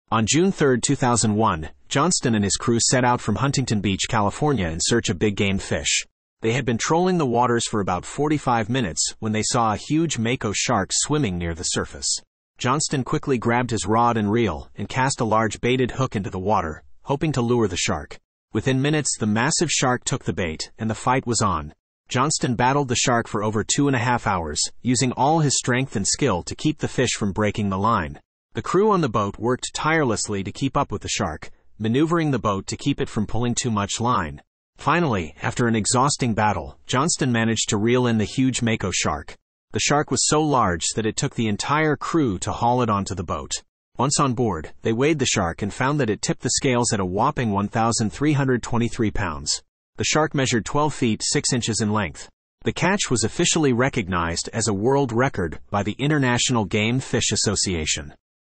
Angler Reels in Monster Mako Shark Sets World Record
The story of the world record catch of a Mako shark.